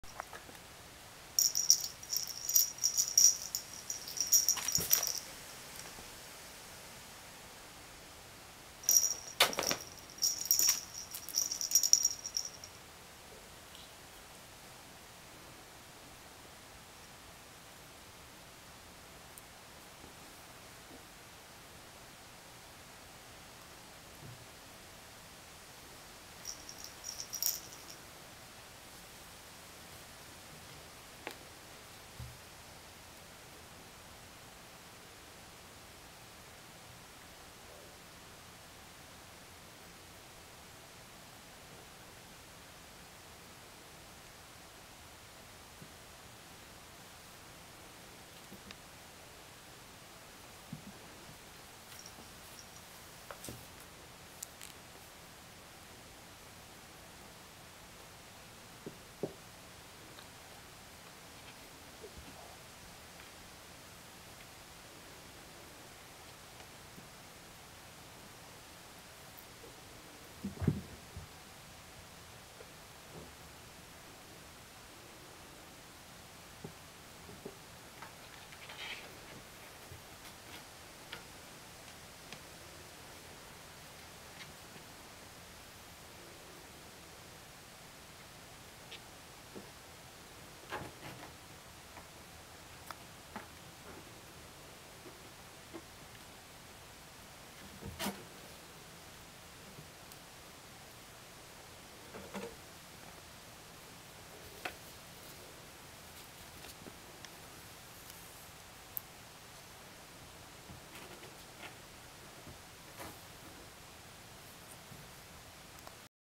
Balinese kittens playing
15 week old Balinese kittens (girls) playing with a toy.